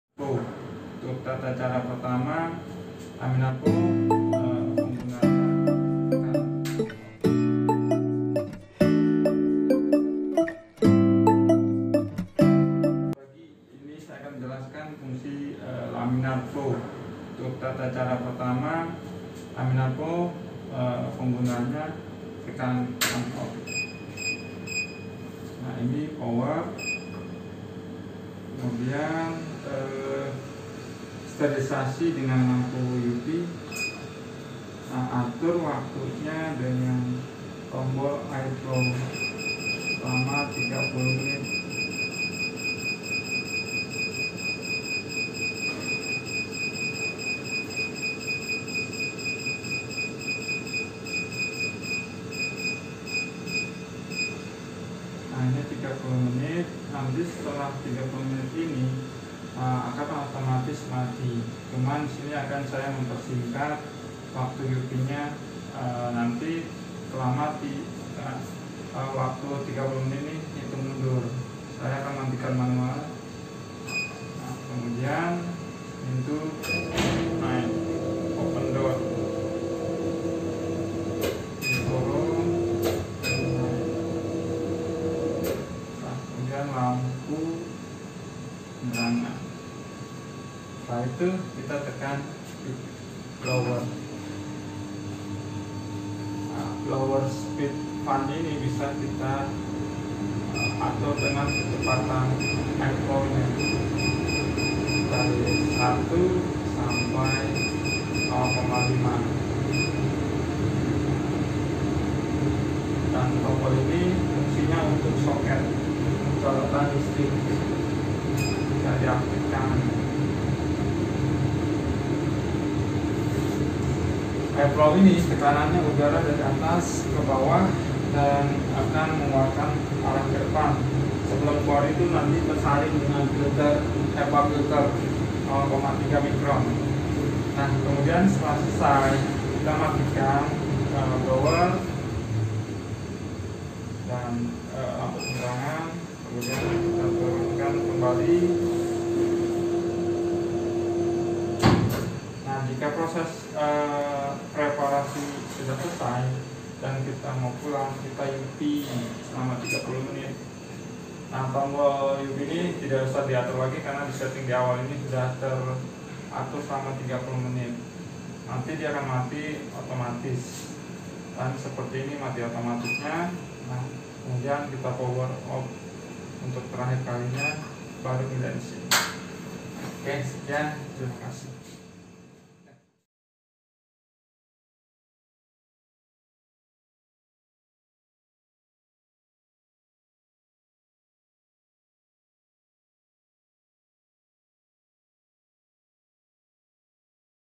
Cara Penggunaan Laminar Flow Cabinet BBS V1300 BIOBASE
Laminar Air Flow adalah alat yang dapat mengalirkan udara steril secara terus-menerus melewati tempat kejadian, sehingga tempat untuk melakukan implikasi akan terbebas dari debu, spora-spora, dan kotoran yang tidak diinginkan untuk jatuh ke dalam medium.

CARA SETTING JAM BIOSAFETY CABINET